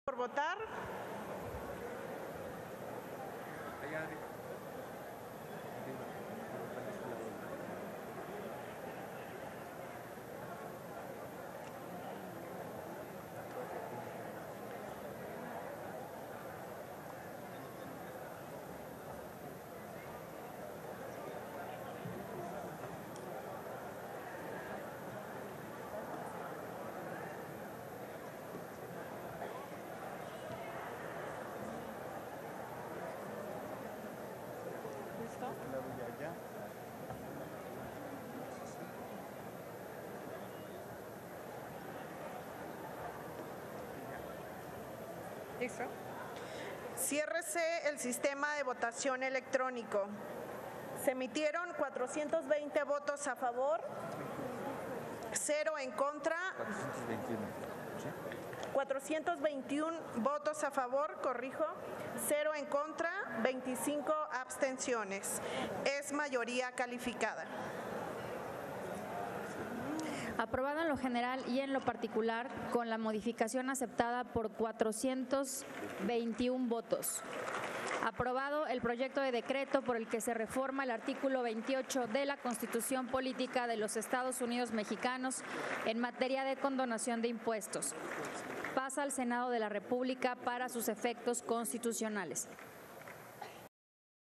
Votación en lo general y particular el dictamen en materia de condonación de impuestos
421 votos a favor, 0 en contra y 25 abstenciones. Avalan, en lo general y en lo particular, el dictamen que reforma el artículo 28 de la Constitución Política, en materia de condonación de impuestos. Se turnó al Senado de la República para sus efectos constitucionales.